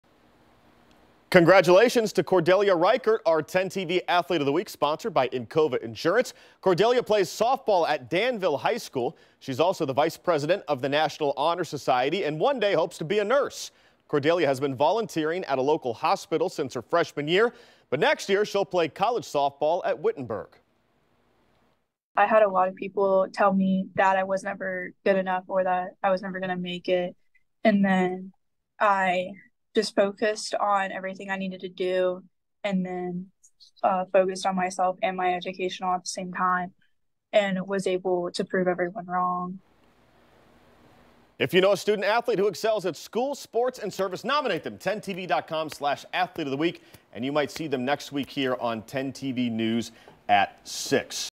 Athlete of the Week: Cordelia Richert
Cordelia Richert plays softball at Danville High School and is the vice president of the National Honor Society.